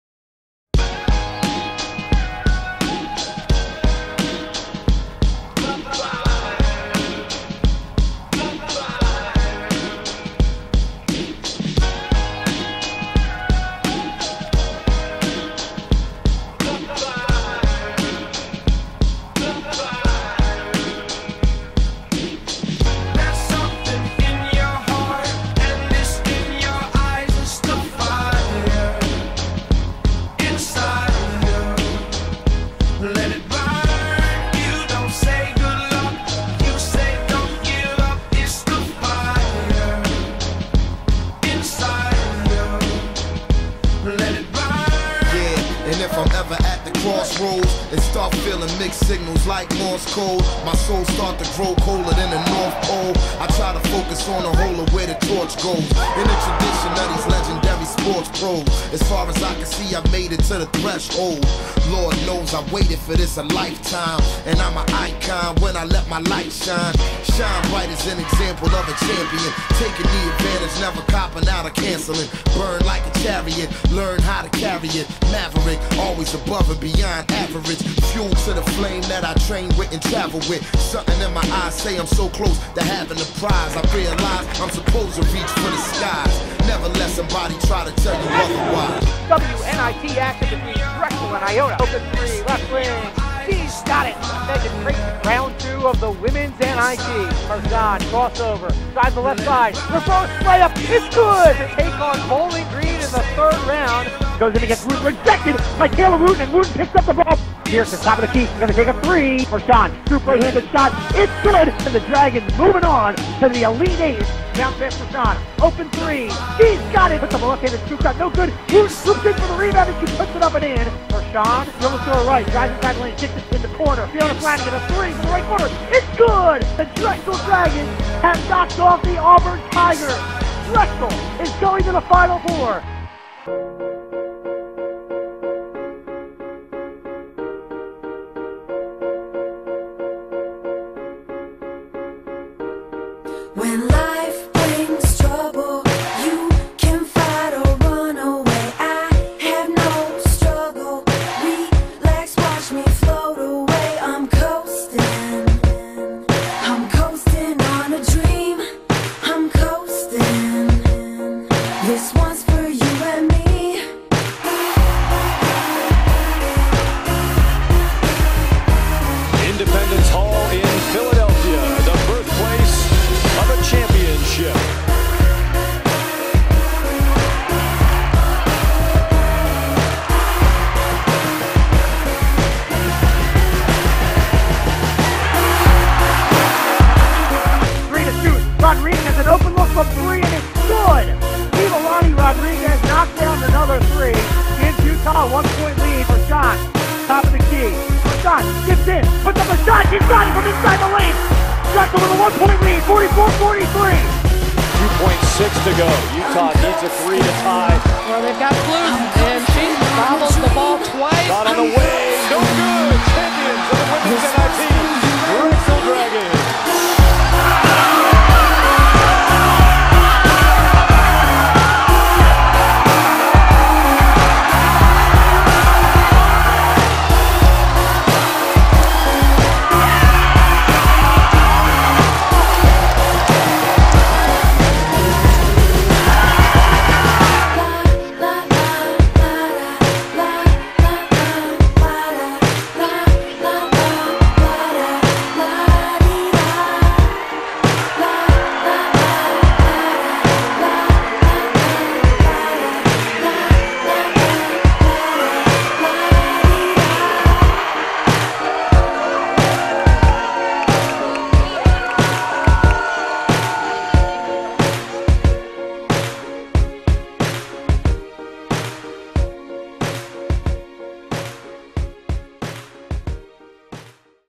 2012-13 Women's Basketball Banquet Video
I created this video for our 2012-13 Drexel Basketball banquet.  This was an important piece to celebrate Women's basketball winning the WNIT Championship.  I edited and produced this video.